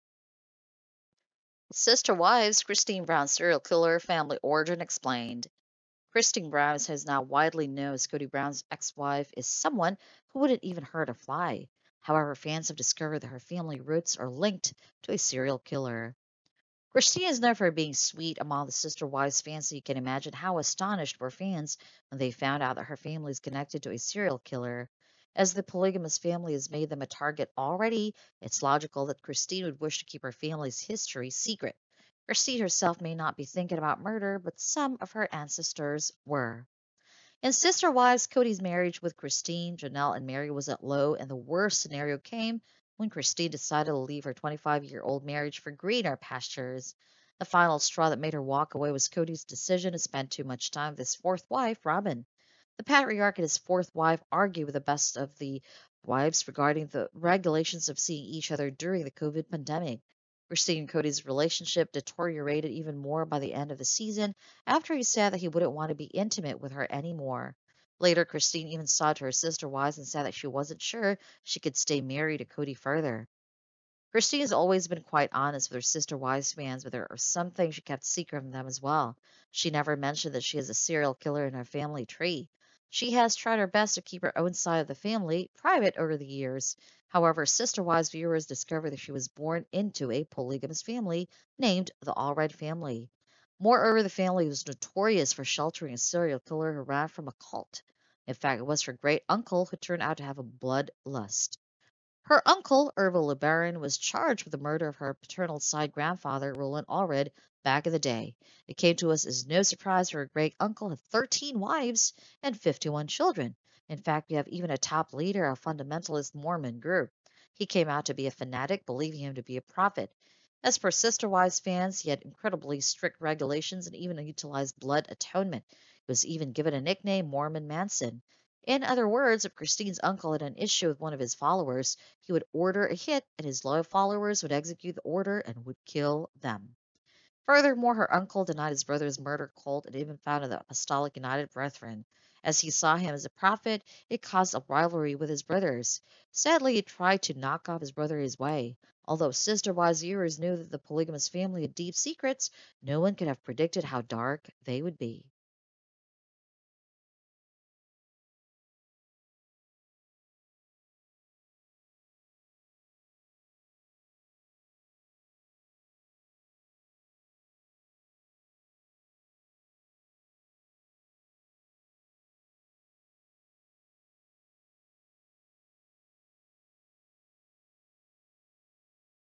Sister Wives: How Christine Brown Shocking Unknown Familial Links to Serial Killers
Christine is known for being sweet among the Sister Wives fans, so you can imagine how astonished were fans when they found out that her family is connected to a serial killer. As the polygamous family has made them a target already, it’s logical that Christine would wish to keep her family’s history secret. Christine herself may not be thinking about murder, but some of her ancestors were.